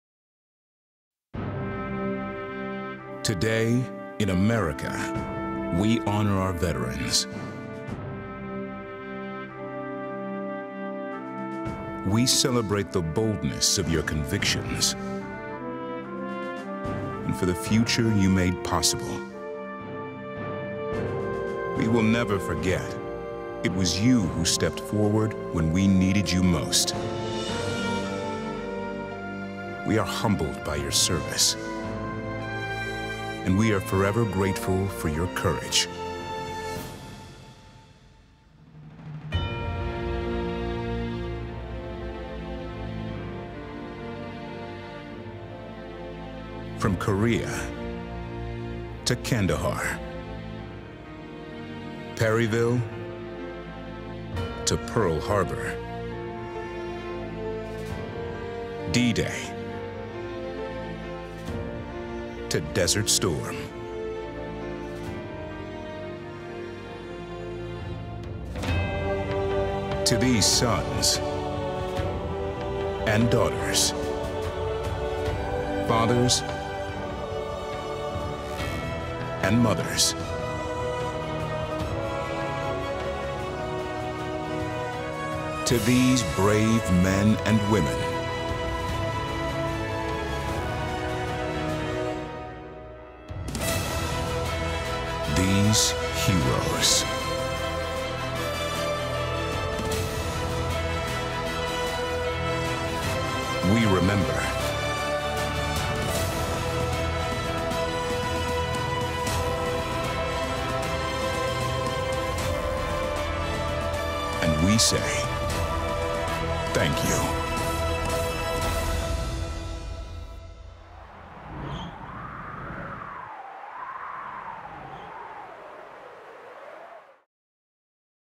WWE pays tribute to all who served on Veterans Day
WWE thanks and honors all the brave men and women who have fought for freedom with a special Veterans Day message.
Follow WWE on YouTube for more exciting action!